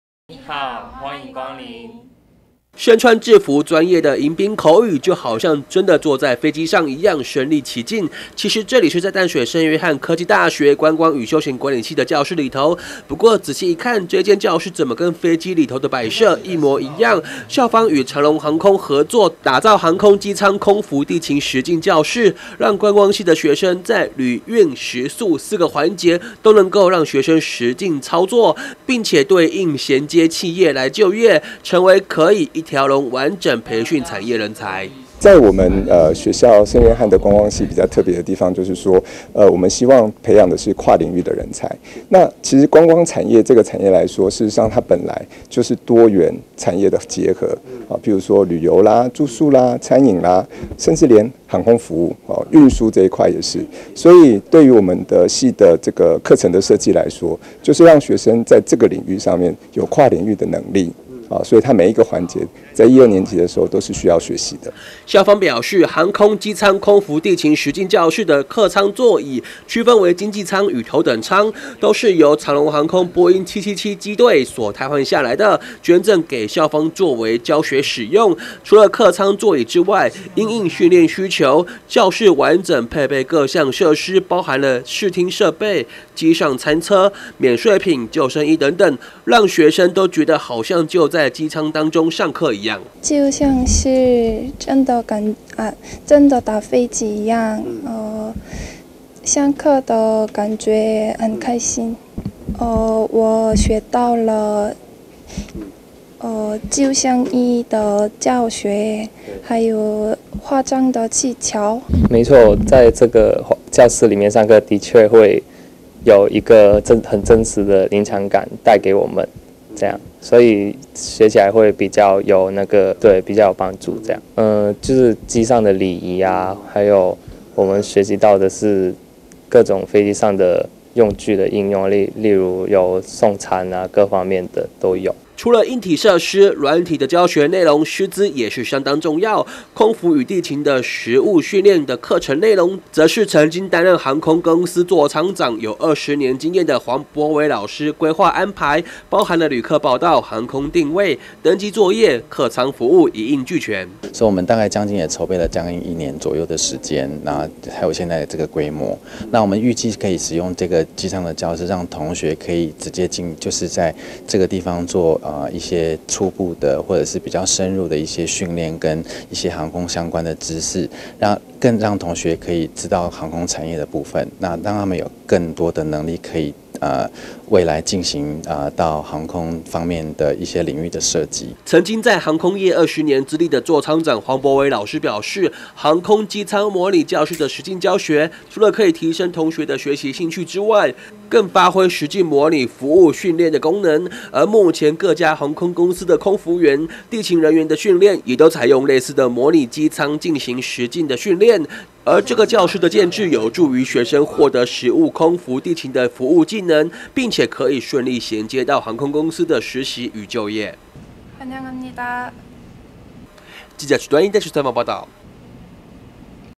1060615 除役設備捐聖約大 觀光系模擬機倉學習更真實
淡水聖約翰科技大學與長榮航空合作，打造航空機艙空服地勤實境教室，讓觀光系的學生，在「旅、運、食、宿」四個環節，都能夠讓學生實境實作，並對應銜接企業就業，成為可一條龍完整培訓產業人才。